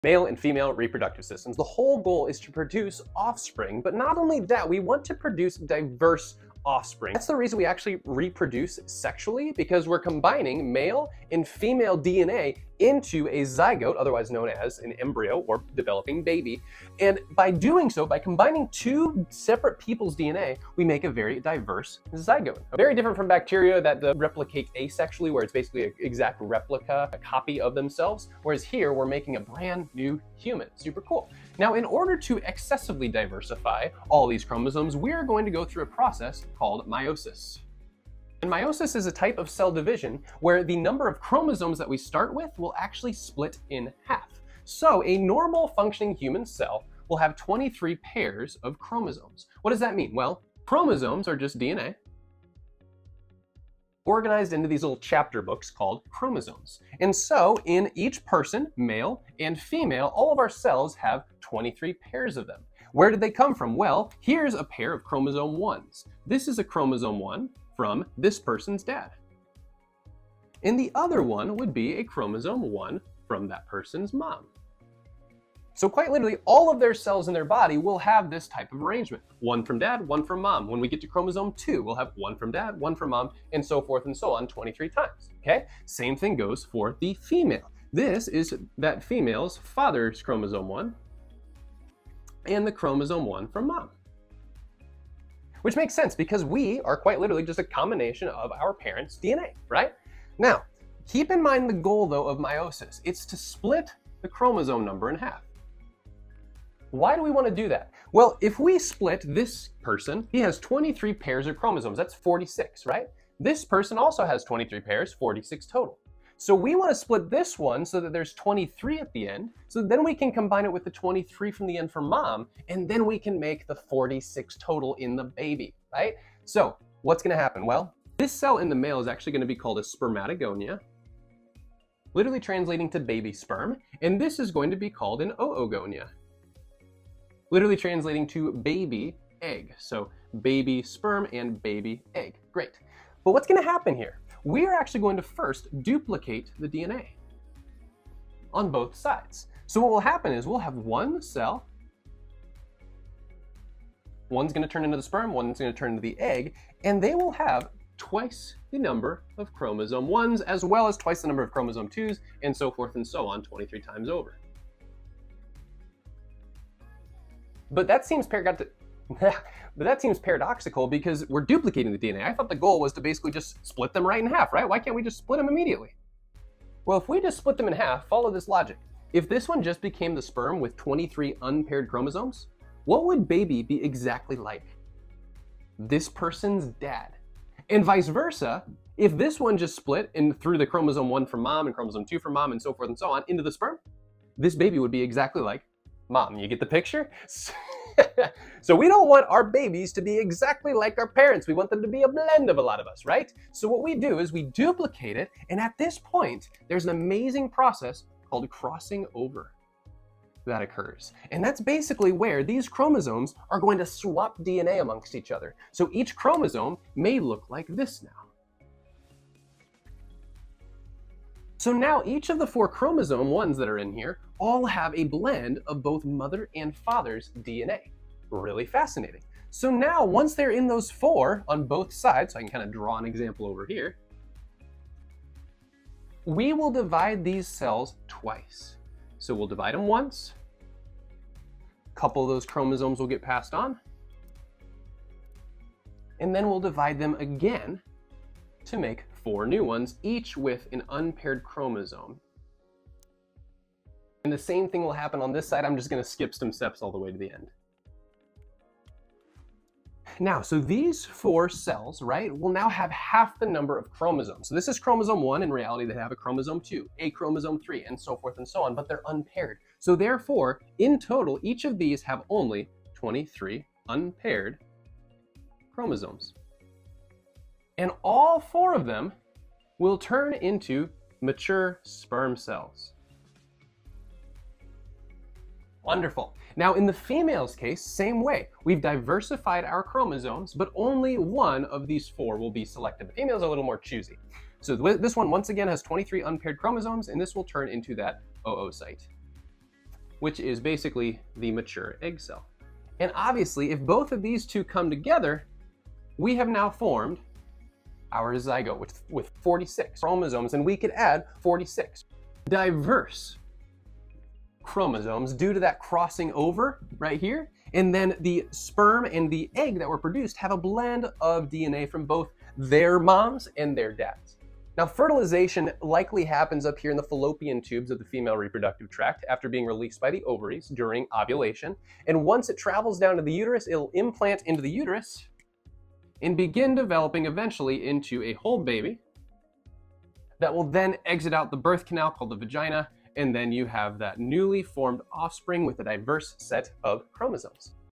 Human Sexual Reproduction - Meiosis (5 Minutes!)
Why do some kids look EXACTLY like their parents, while others look like, say, grandma or grandpa? In this final video of anatomy and physiology as an overview, we look at meiosis and how it provides the offspring with a diverse set of DNA to (hopefully!) survive and thrive!